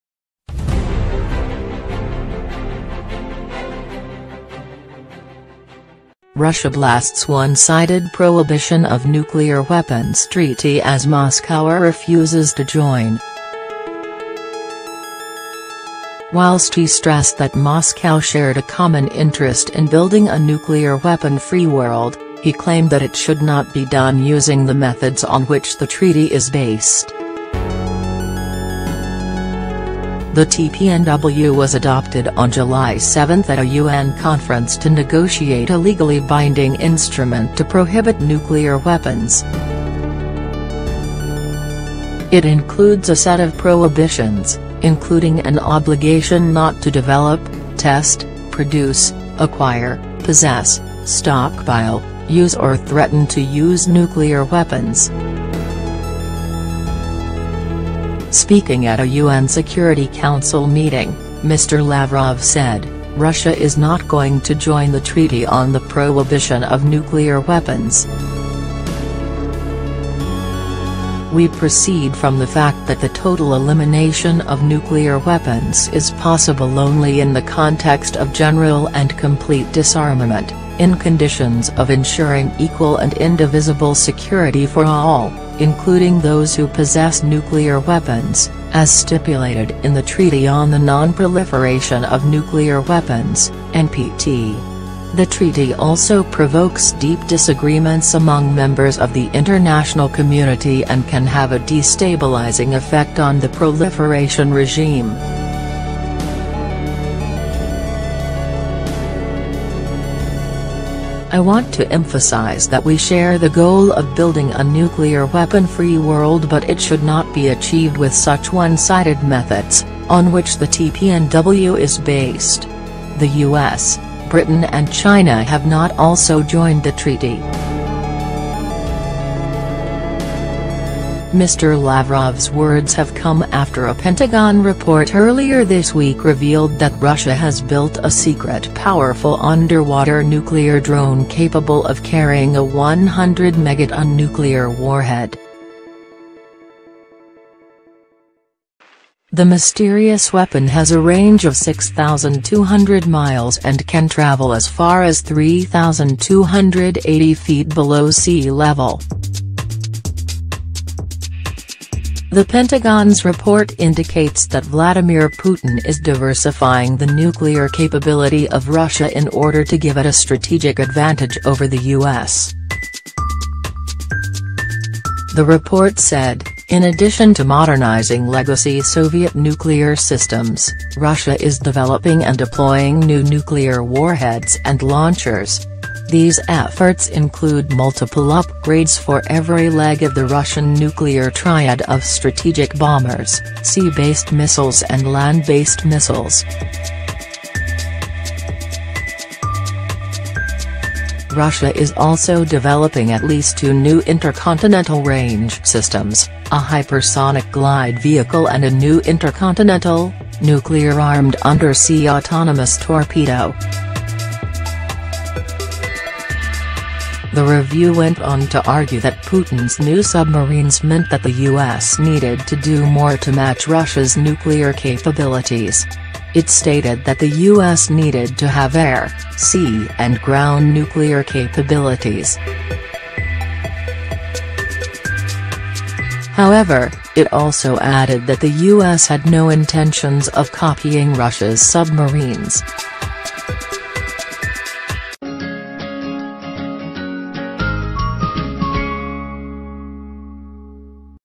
World War 3 news: Russia does not plan to join the UN Treaty on the Prohibition of Nuclear Weapons
Russia blasts 'one sided' prohibition of nuclear weapons treaty as Moscow REFUSES to join